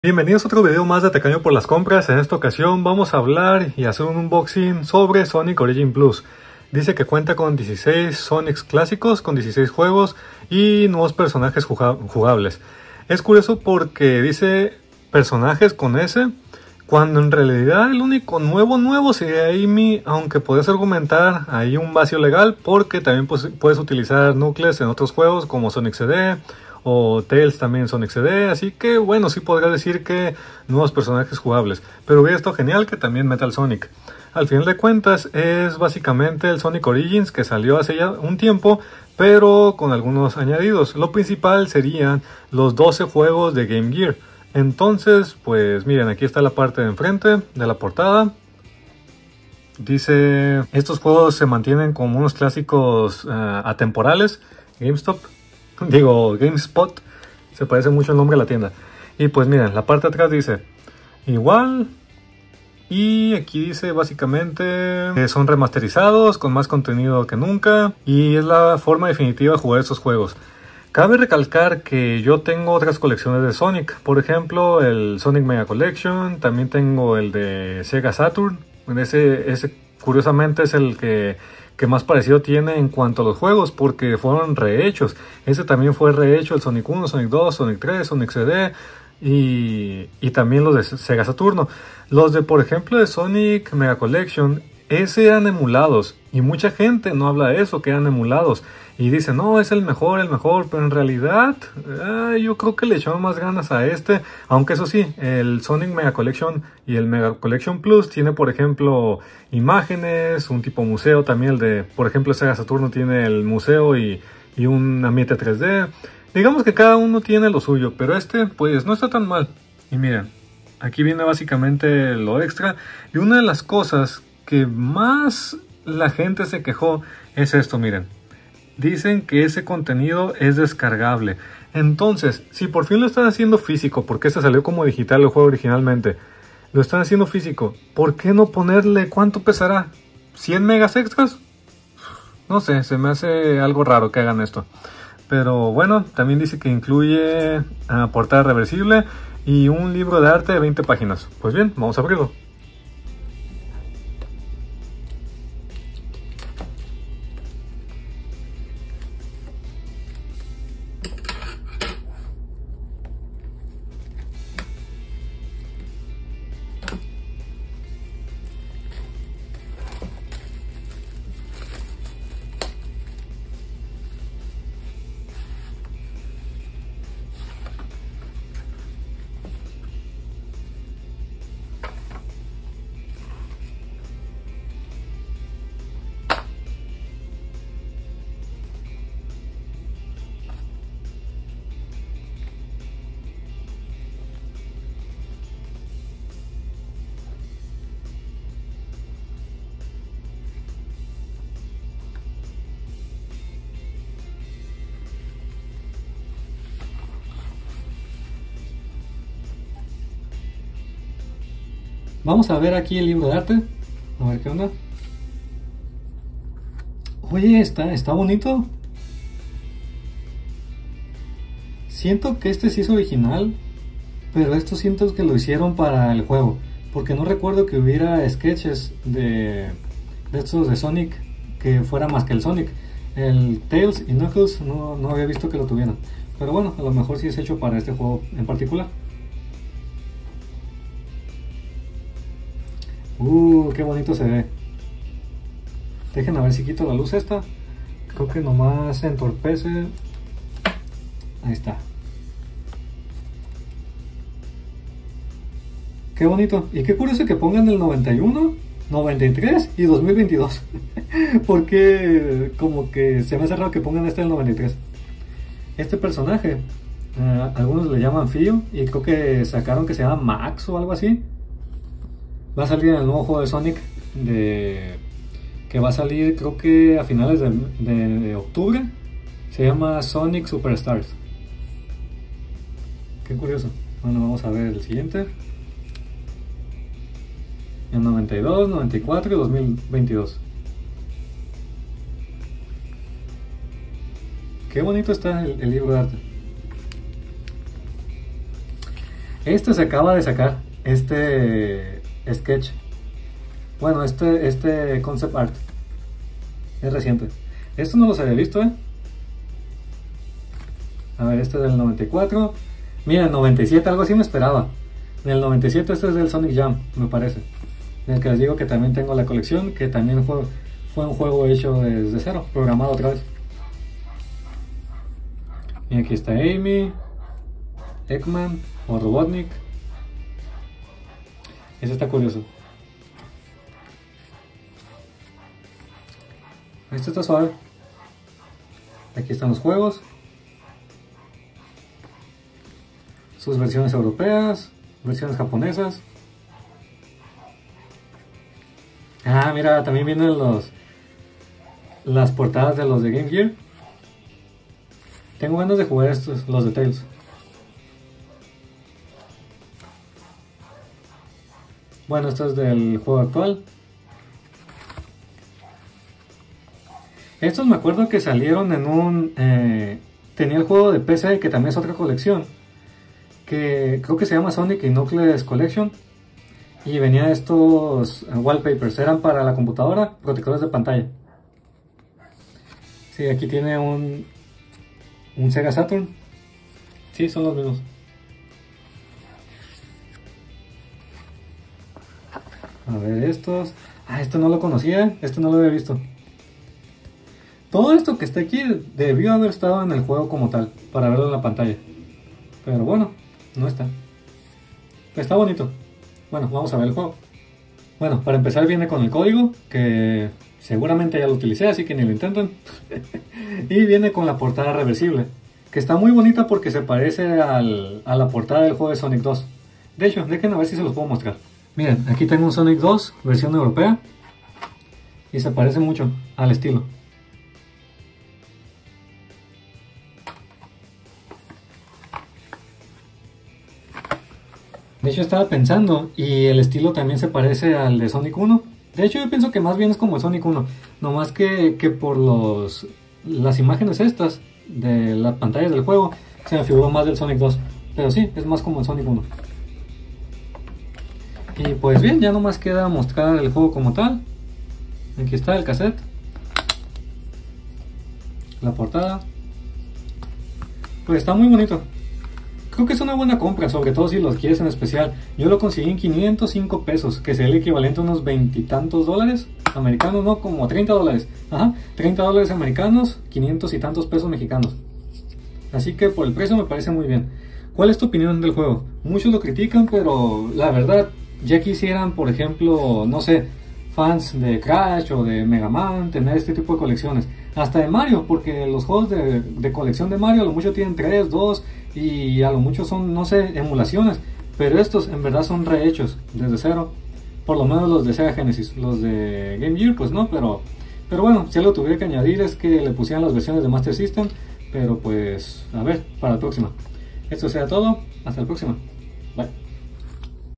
Sonic Origin Plus Versión Nintendo Switch con Libro de Arte
Sonic Origins Plus es una de las mejores recopilaciones de juegos de Sonic The Hedgehog, incluye los 4 títulos principales de Sega Genesis y 12 juegos emulados de Sega Game Gear además de añadidos como Amy clásica jugable por primera vez en Sonic CD
🎁 Adquiere un Año de Hosting para crear tu propia pagina web con Dominio y SSL Gratuito en Hostinger